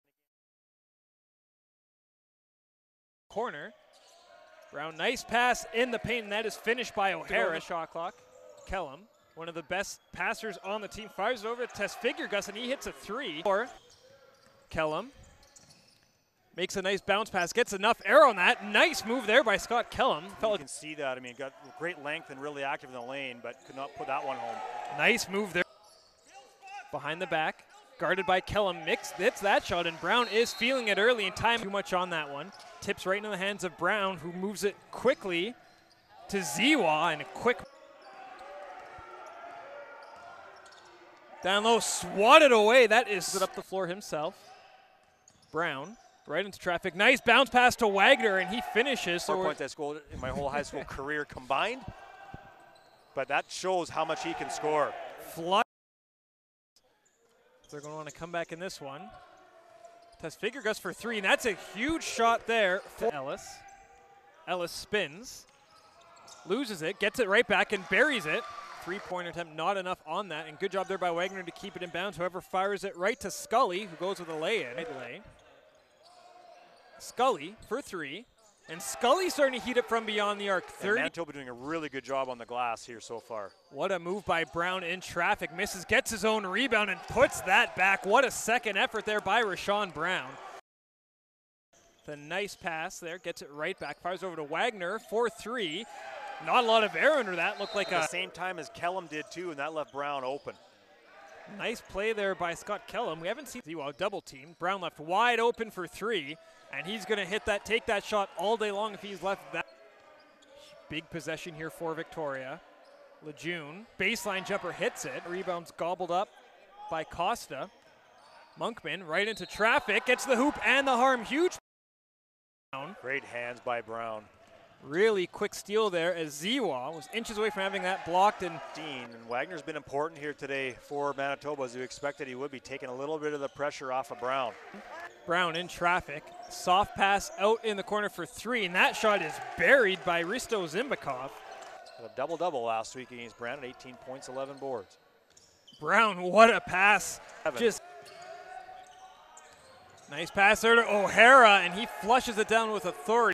MBB: VIC (61) vs. MAN (81)
The Manitoba Bisons beat the Victoria Vikes in the Canada West men's basketball quarterfinal on Saturday, February 22 in Edmonton, Alberta. They advance to the Canada West semifinal where they'll take on the Alberta Golden Bears